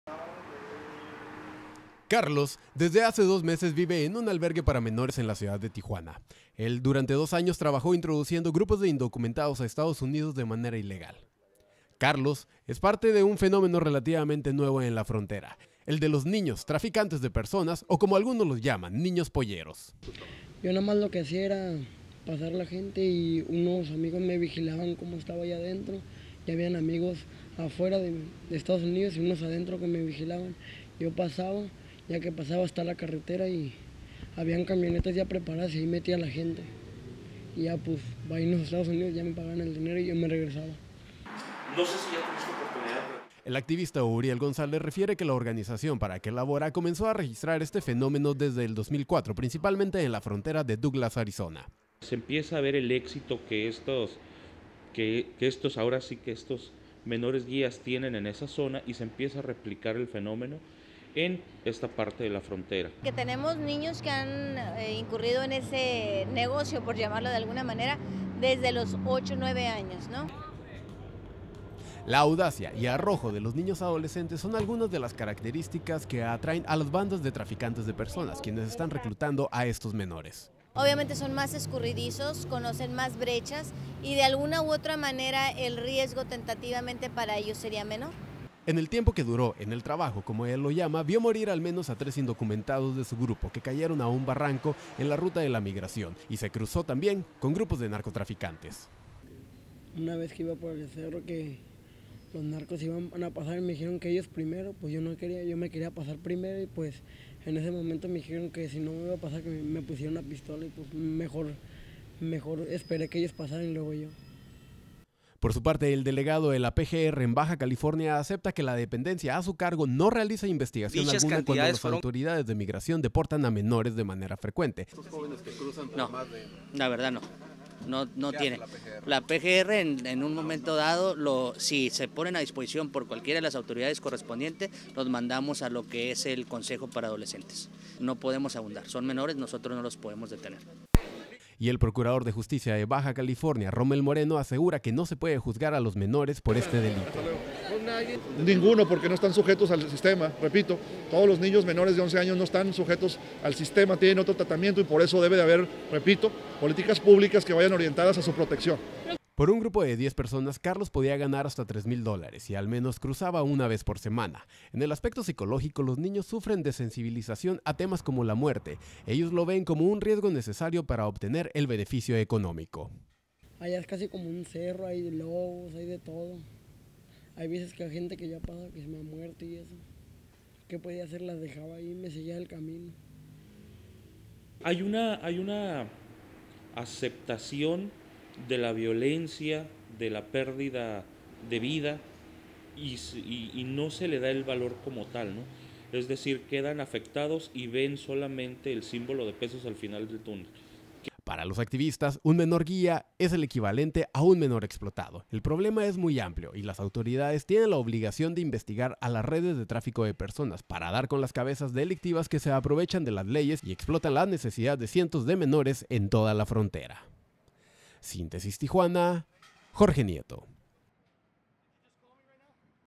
Niños Polleros
"A veces si hay peligro, una vez unos narcos me pusieron la pistola en la cabeza,
 otro dia un migrante se murió en el camino y pues ahí lo dejé, yo que podia hacer" Son palabras de un niño que durante dos años fue utilizado para traficar indocumentados de México a Estados Unidos.